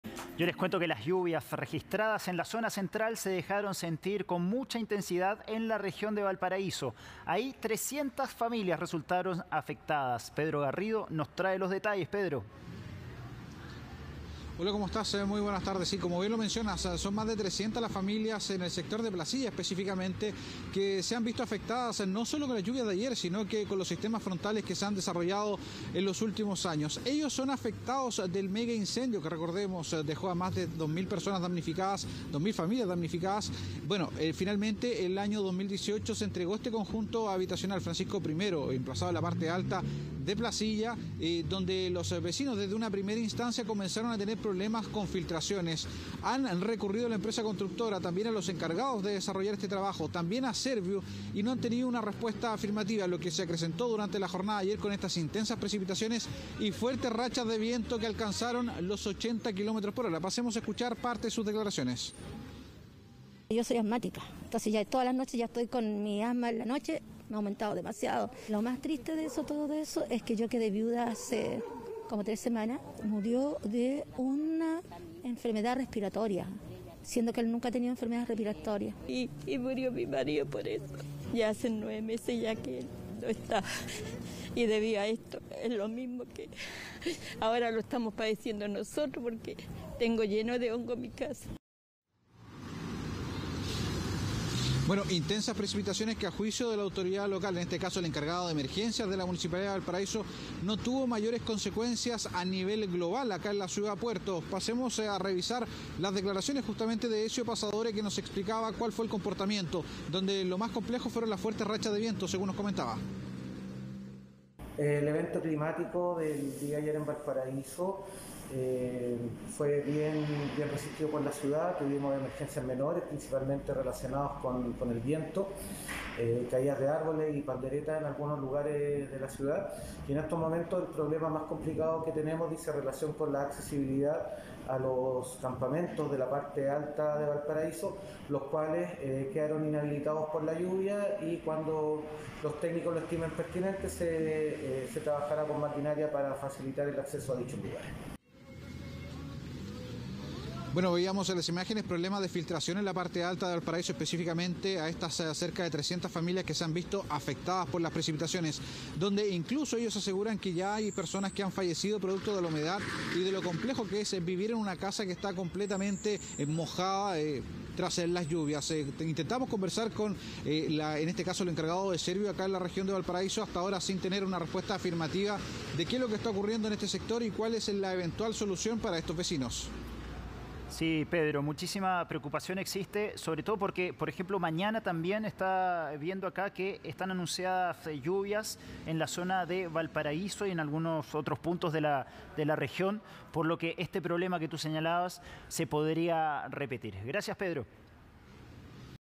300 familias afectadas por las lluvias en la región de Valparaíso
Las familias que viven en el conjunto habitacional Francisco I, en la localidad de Placilla, se han visto afectadas desde hace varios meses por filtraciones, una situación que se acrecentó tras el sistema frontal que se registró esta semana.